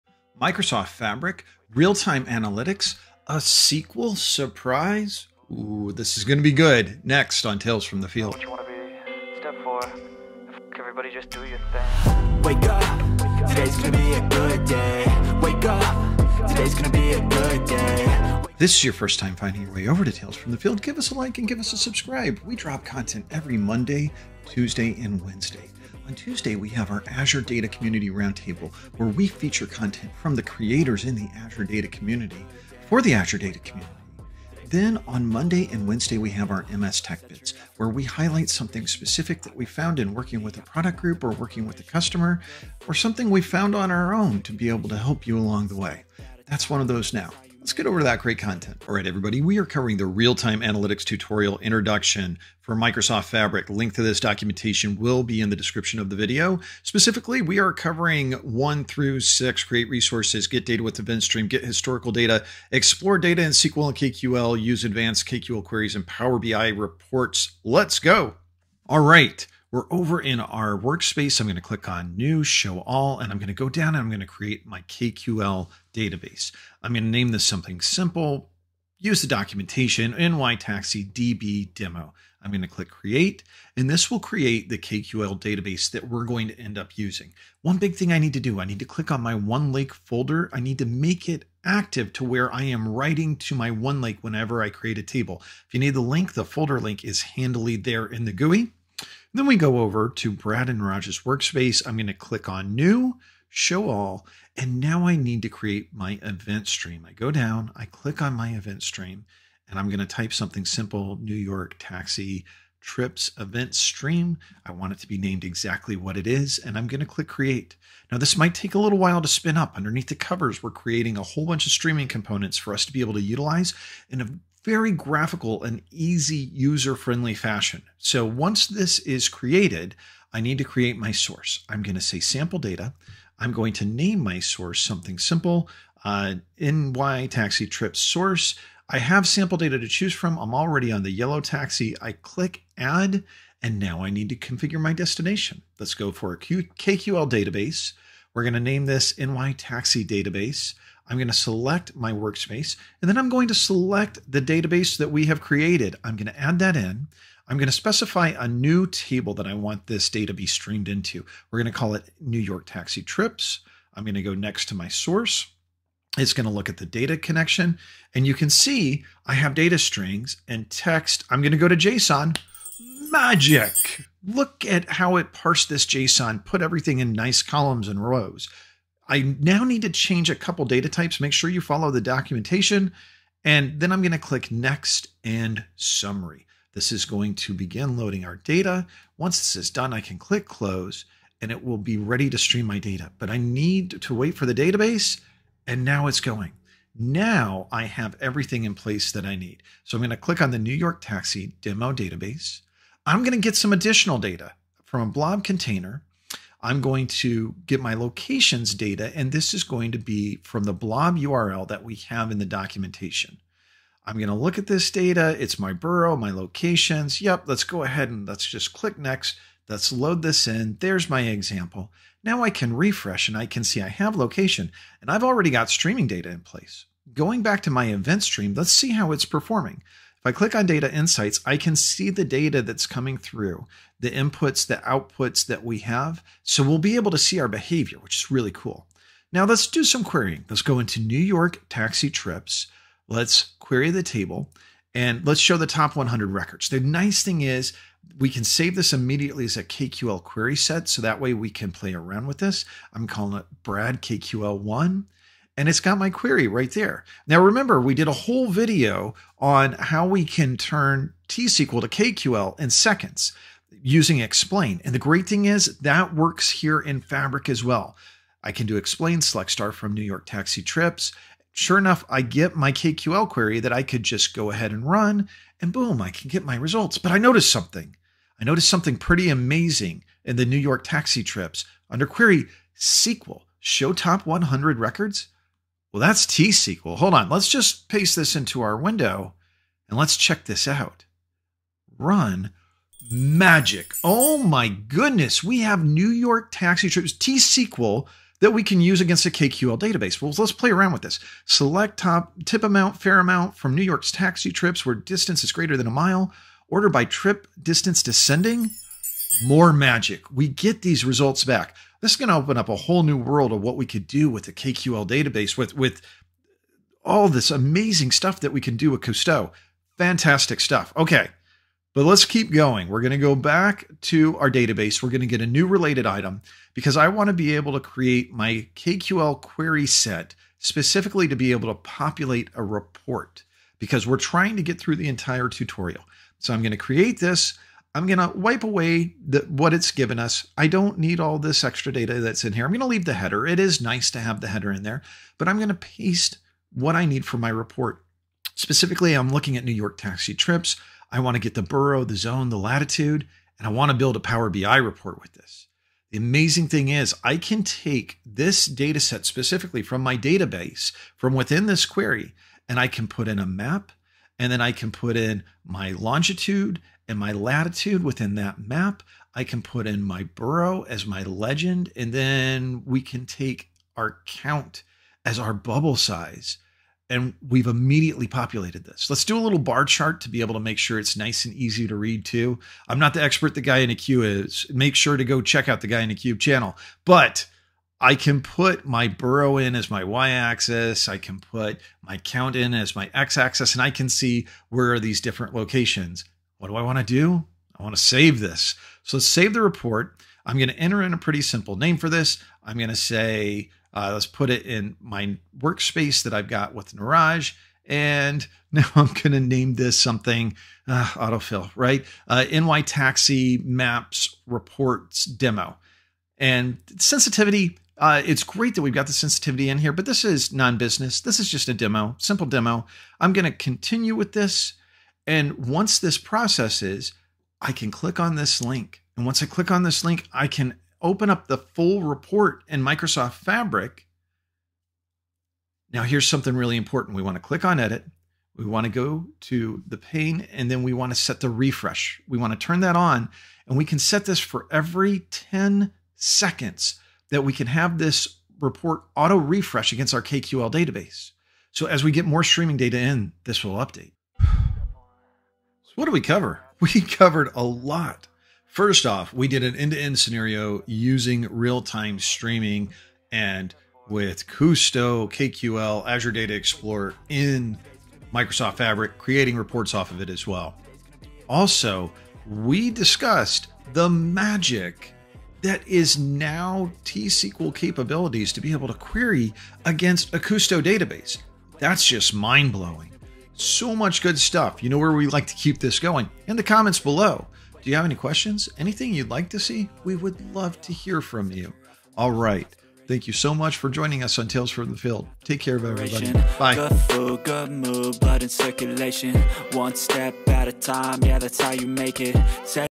Microsoft Fabric Real-Time Analytics Tutorial!!
@SQLBalls walks through how to create a Real-Time Analytics architecture in MicrosoftFabric. This follows the current documentations online. Keep a look out for the SQL Magic!!

Chapters
00:00 Introduction
00:49 WHAT ARE WE DOING!?!
01:09 Demo 1 Create Resources
01:41 Demo 2 Create Eventstream
03:16 Demo 3 Get historical Data
04:02 Demo 4 Explor data with KQL & SQL
05:21 Demo 5 Use advanced KQL queries
05:51 Demo 6 Build a Power BI Report
07:35 The WRAP UP (did you miss the magic?)

Real-Time Analytics tutorial part 2: Get data with Eventstream

Real-Time Analytics tutorial part 4: Explore your data with KQL and SQL

Real-Time Analytics tutorial part 5: Use advanced KQL queries

Real-Time Analytics tutorial part 6: Build a Power BI report